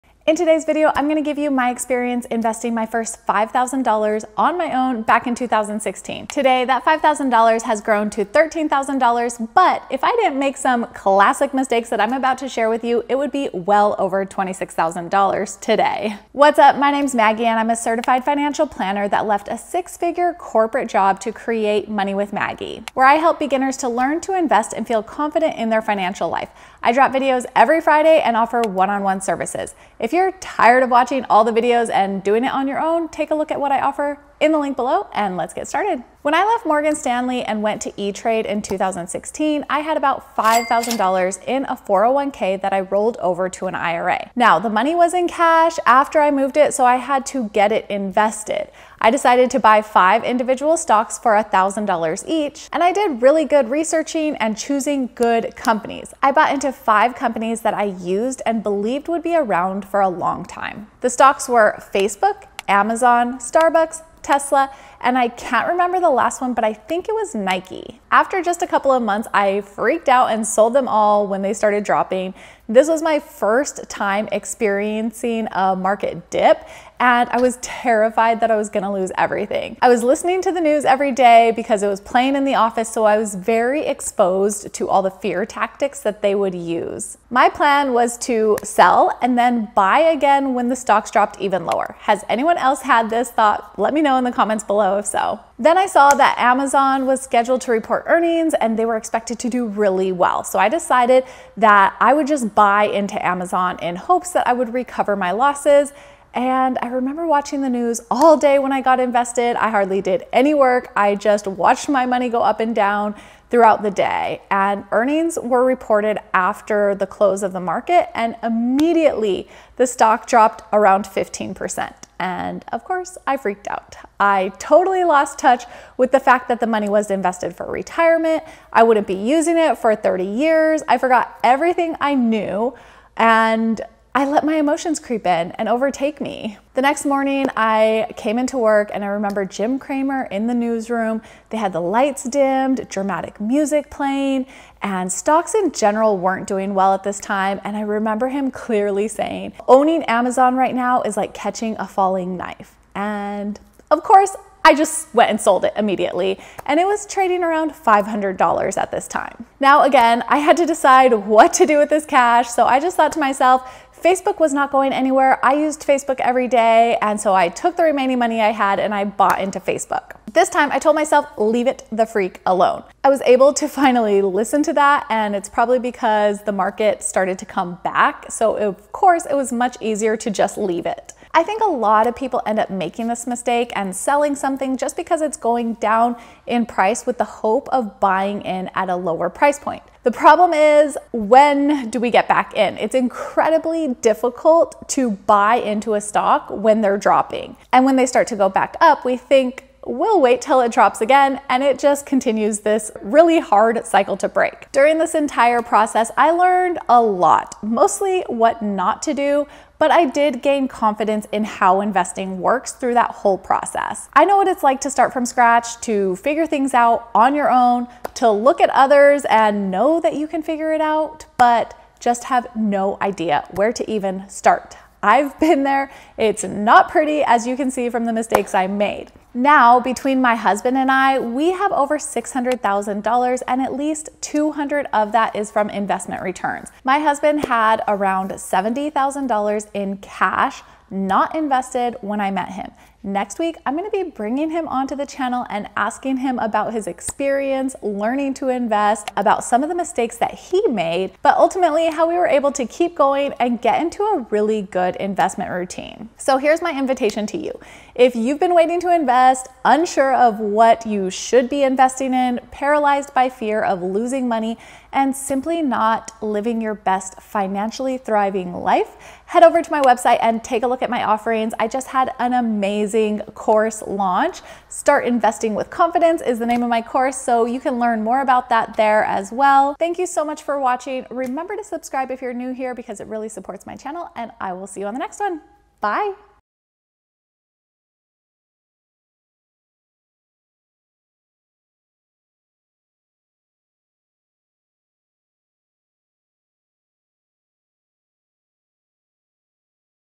Lessons learned investing my first $5,000 🤦🏽‍♀️
In today's video, I'll give you my experience investing my first $5k on my own back in 2016. Today that 5k has grown to $13k, but if I didn't make some classic mistakes that I'm about to share with you, it would be well over $26k today.

⭐️ If you need help creating your own financial plan, sign up for a 1:1 session

💻 Join my Investing for beginners course waitlist (Next Cohort - Early Spring 2023)